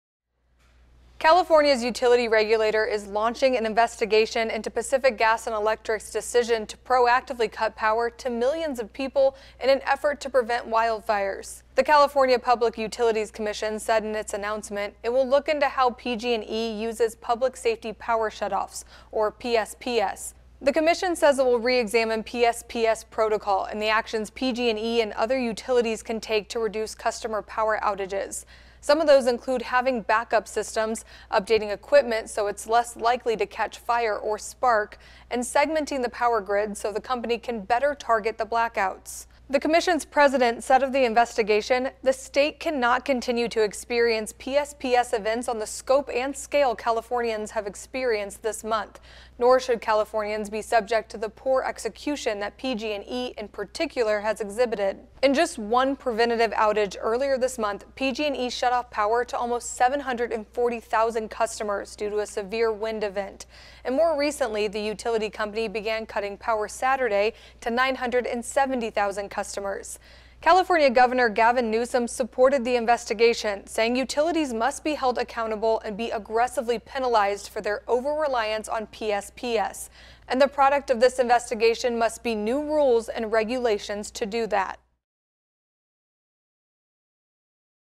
Calif. utility regulator to investigate PG&E
California Gov. Gavin Newsom called for the investigation into PG&E's use of Public Safety Power Shutoffs.